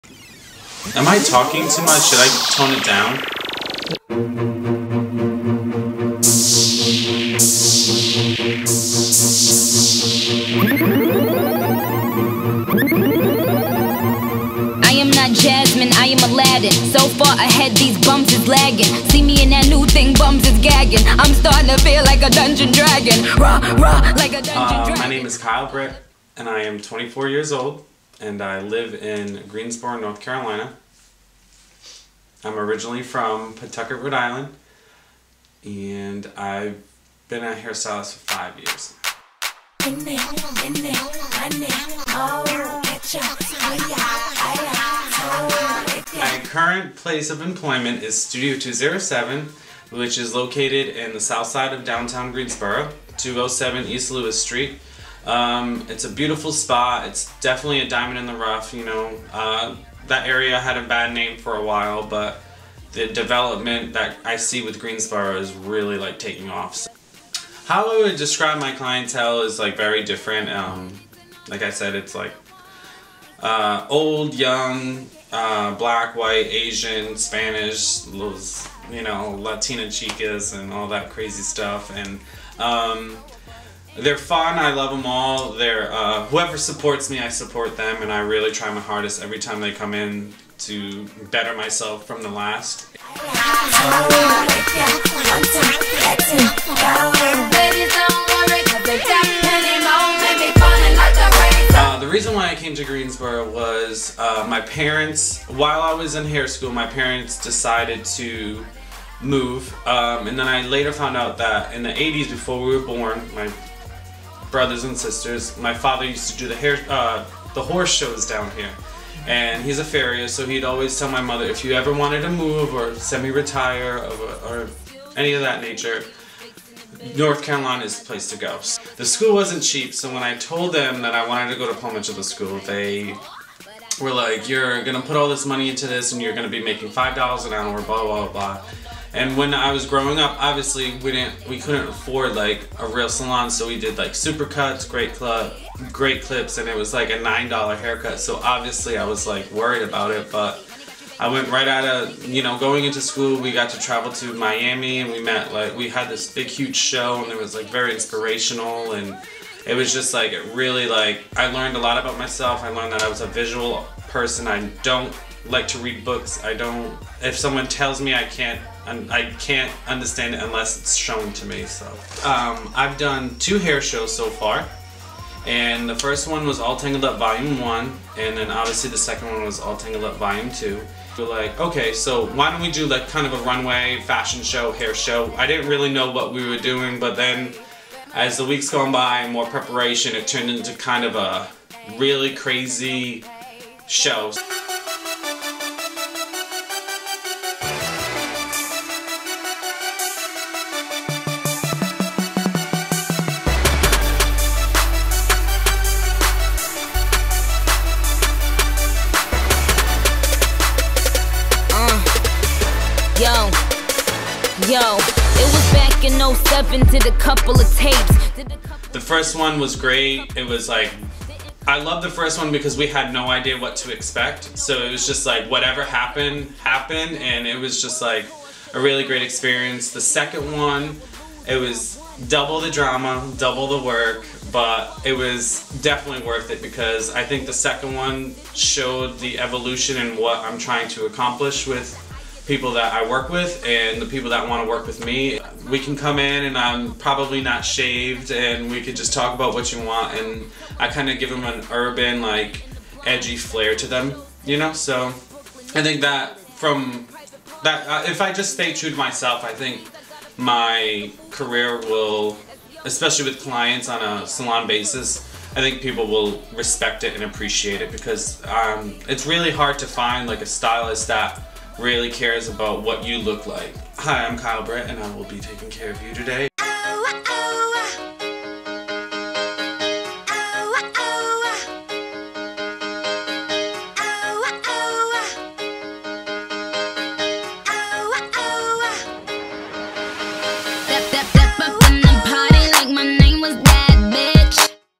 A Cut Above the Rest - Kyle Britt
This is a short Documentary I did for a class. It could only be 5 minutes. I did it on the talented Kyle Britt. A hair stylest in Greensboro, NC. I do not take any credit for the photography as they are claimed by their respective photographers. They are showcased to show the creative work of Kyle Britt.